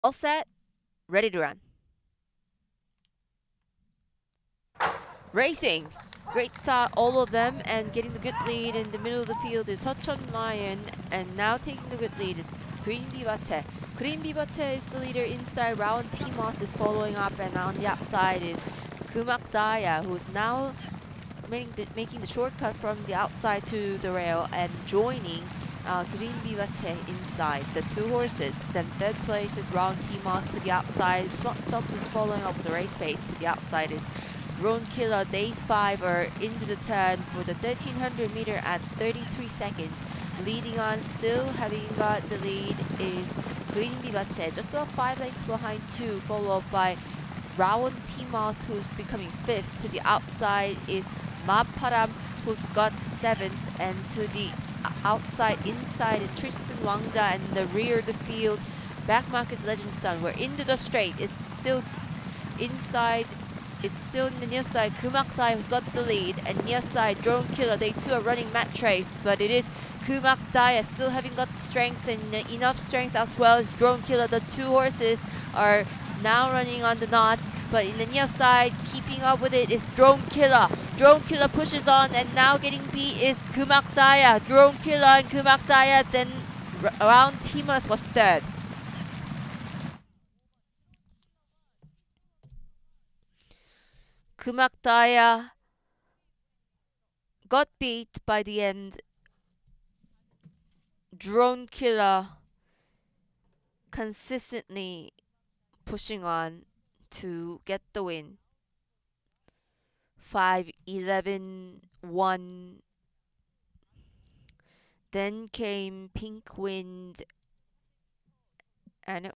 (Seoul) 2024.07.28 R9 Class 4 (1300M) Handicap
W: DRONE KILLER (KOR)
J: CC Wong
T: Park Jae-woo
O: Lee Kwan-hyeong